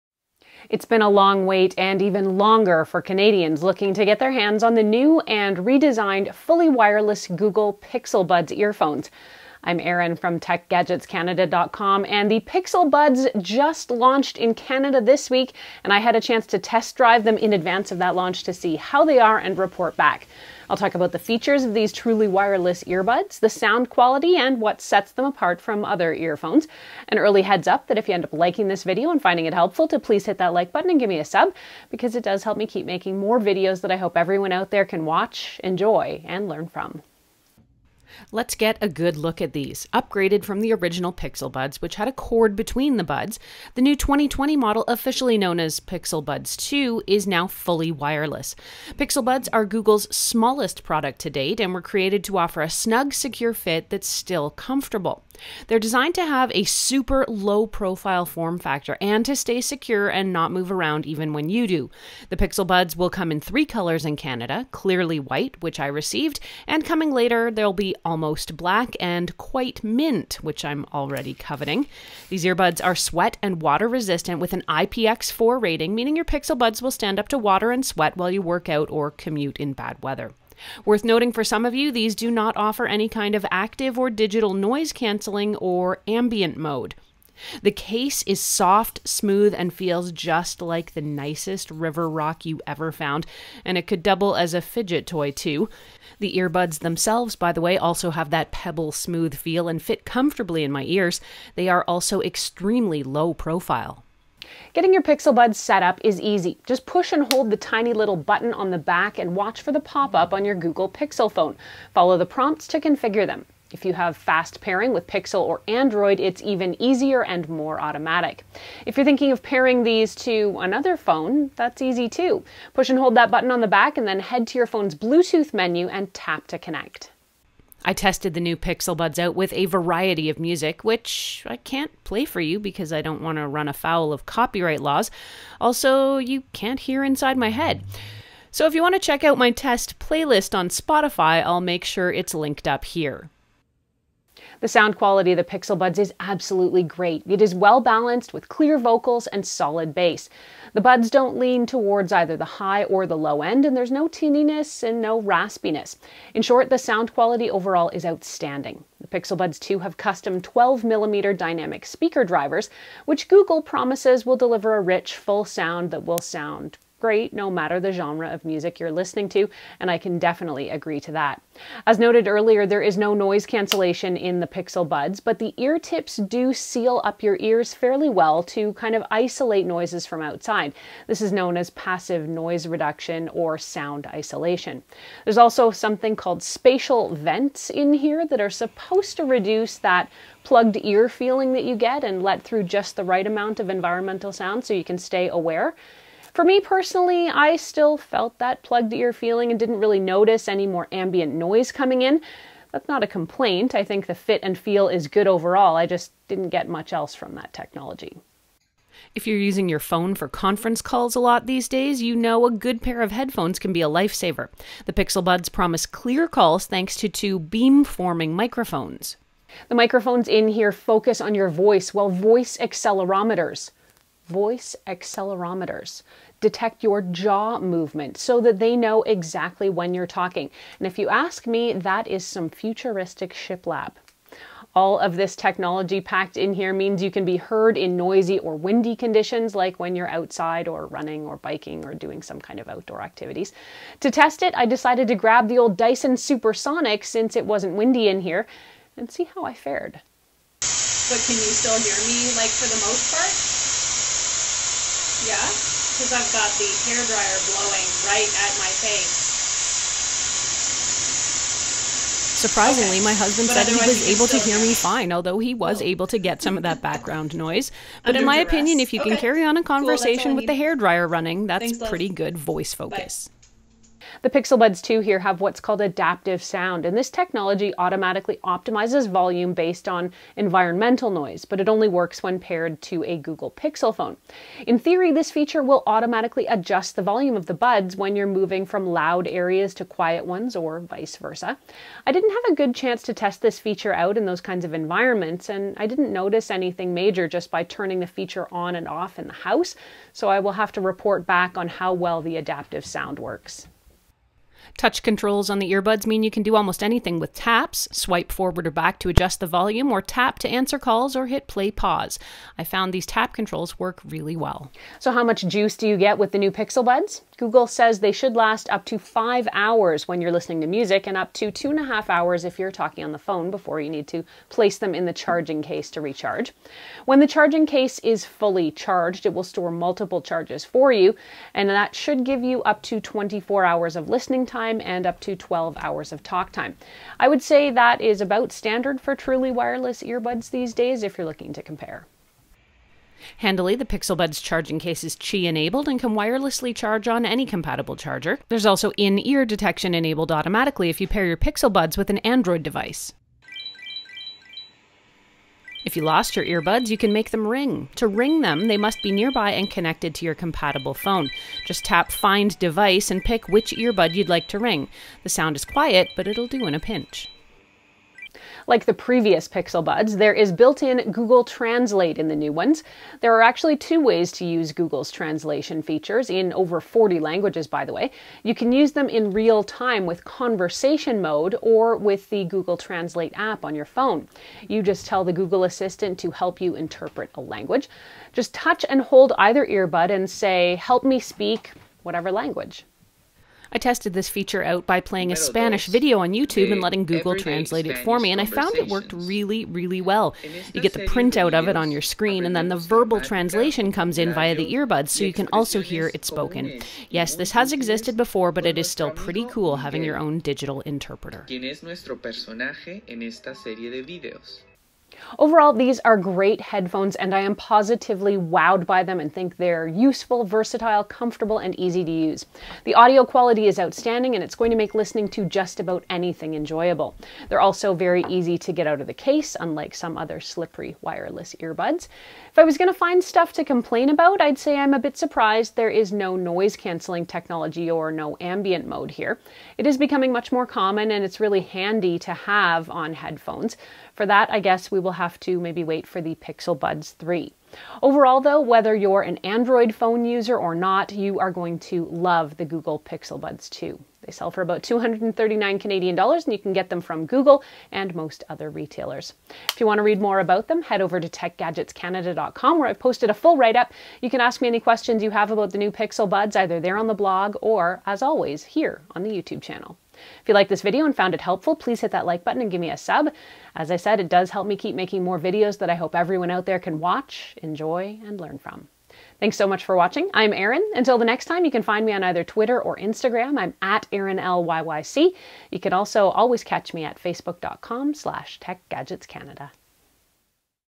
New Pixel Buds 2 review 2020
It’s been a long wait, and even longer for Canadians looking to get their hands on the new and redesigned fully wireless Google Pixel Buds earphones.  the Pixel Buds just launched in Canada this week and I had a chance to test drive them in advance of that launch to see how they are and report back. I’ll talk about the features of these truly wireless earbuds, the sound quality and what sets them apart from other earphones.

Let’s take a good look at these. Upgraded from the original Pixel Buds which had a cord between the buds, the new 2020 model officially known as Pixel Buds 2 is fully wireless.

These earbuds are sweat and water resistant with an IPX4 rating, meaning your Pixel Buds will stand up to water and sweat while you work out or commute in poor weather.

Worth noting for some of you is that these do not offer any kind of active or digital noise cancelling or ambient mode.

If you’re pairing these to another phone, that’s easy too. Push and hold that button
then head to your phone’s Bluetooth menu then tap to connect.

I tested the new Pixel Buds out with a variety of music… which I can’t play for you because I don’t want to run afoul of copyright. Also, you can’t hear inside my head. If you want to check out my test playlist on Spotify it’s linked up here.

The sound quality is absolutely great. It’s well balanced with clear vocals and solid bass. The Pixel Buds don’t lean towards either the high or low end, there’s no tinniness or rasp to them. In short, the sound quality is outstanding.

Pixel Buds 2 have custom 12mm dynamic speaker drivers which Google promises delivers a rich, full sound that sound great no matter the genre and I can definitely agree to that.

As noted earlier, there’s no noise cancellation but the Pixel Buds’ eartips seal your ears to isolate loud outside noises (this is known as passive noise reduction).

There’s also something called spatial vents that are supposed to reduce that plugged-ear feeling, and let through just the right amount of environmental sound so you can stay aware.

For me personally, I still felt that plugged ear feeling and I didn’t notice any more ambient noise coming in. That’s not a complaint; I think the fit and feel is good overall, I just don’t get much else from that technology.

All this technology means you can be heard in noisy and windy conditions, like when you’re running or biking.

The Pixel Buds 2 have what’s called Adaptive Sound. This technology automatically optimizes volume based on environmental noise, but it only works when paired to a Google Pixel Phone. In theory this feature will automatically adjust volume for you when you’re moving from loud areas to quiet ones.

I didn’t have a good chance to test this feature out in these kind of environments and I didn’t notice anything just turning them off and on  in the house, so I’ll have to report back on how well the Adaptive Sound works

You can control almost anything using taps on the earbuds. Swipe forward or back to adjust volume; tap to answer calls or play/pause. I found these tap controls work very well and are easy to adjust to.

Handily, the Pixel Buds charging case is Qi enabled and can wirelessly charge on any compatible charger.

Also, In-ear detection is enabled automatically if you pair your Pixel Buds with an Android 6.0+ device.

If you misplaced your earbuds, you can make them ring. To ring your earbuds, they must be nearby and connected to your compatible phone.
○ Tap Find device  Pick which earbud you’d like to ring. The sound is quiet, but it’ll do in a pinch.

Like the previous Pixel Buds there’s built in Google translate in the new Pixel Buds.

Overall, these are great headphones! I’m positively wowed by them and think they’re useful, versatile, comfortable and easy to use. The audio quality is outstanding and will make listening to just about anything enjoyable.

They’re also very easy to get out of the case, unlike some other slippery wireless earbuds.

If I was going to find stuff to complain about, I’d say I’m a bit surprised there’s no noise cancelling technology or no ambient mode in here. It’s becoming much more common and it’s really handy on really good headphones. For that, I guess we’ll wait for Pixel Buds 3…

Overall though whether you’re an Android phone user or not, you’re going to love the Google Pixel Buds 2. They sell for about$239CAD and you can get them from Google